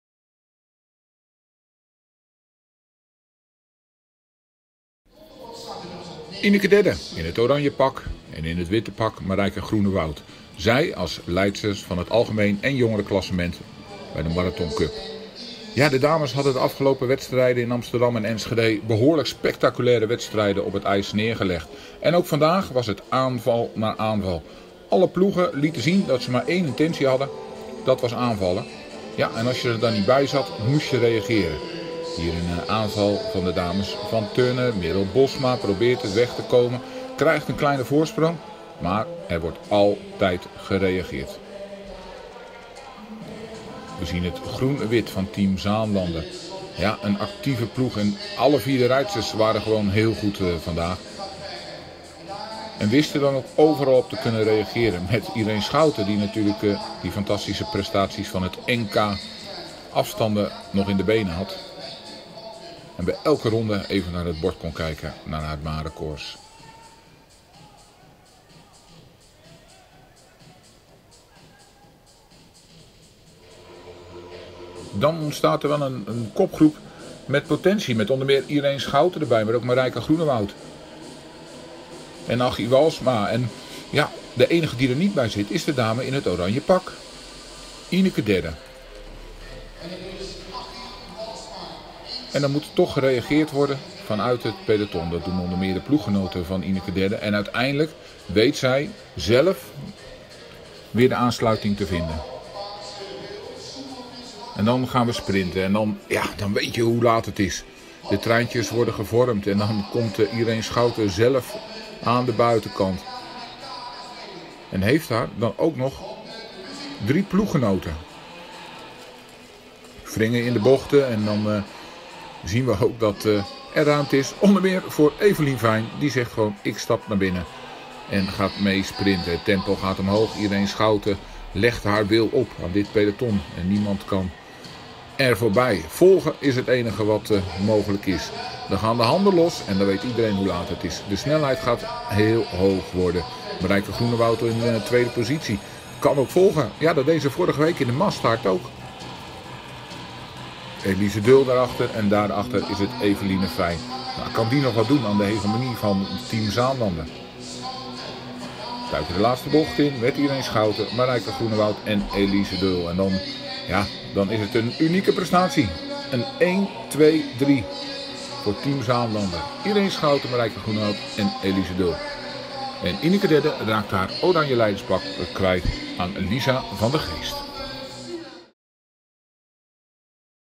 Samenvatting Marathon Cup 3 Heerenveen - Vrouwen
Samenvatting van de live-uitzending op 6 november 2021. De derde Marathon Cup bij de vrouwen van seizoen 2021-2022, Thialf Heerenveen.